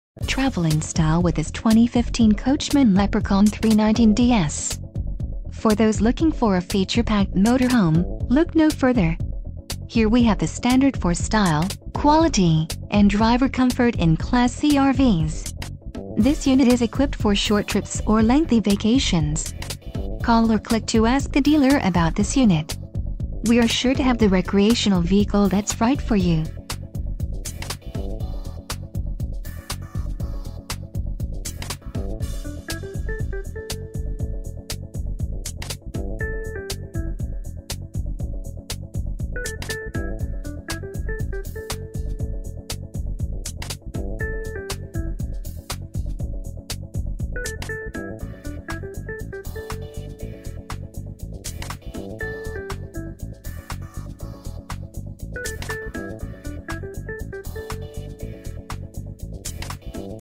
2015 Coachmen Leprechaun 319DS - Alliance Coach FL - Wild...
319DS    Wheel Base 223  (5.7 m)  Gross Vehicle Weight Rating (GVWR) 14,500 lbs. (6,577 kg)  Gross Combined Weight Rating (GCWR) 22,000 lbs. (9,979 kg)  Fuel Capacity 55 gal. (208 L)  Exterior Length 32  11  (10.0 m)  Exterior Height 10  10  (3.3 m)  Exterior Width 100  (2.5 m)  Fresh Water Capacity 50 gal. (189 L)  Gray Water Capacity 32 gal. (121 L)  Black Water Capacity 29 gal. (110 L)  Awning Size 21 ft 0 in.  Accessories: Cognac Interior Color|Ciera Cherry Wood |Full Body Paint|Fire Opal Exterior|Molded Front Cap|Spare Tire|Swivel Front Seat|Swivel Passenger Seat|Exterior Privacy Windshield Cover|Camp Kitchen|Fireplace|Dual Recliners|Hyd Auto Level Jacks|15 BTU Air with Heat Pump|Air Assist Suspension|39 in TV/DVD w/ Elec Lift|Outside Entertainment Center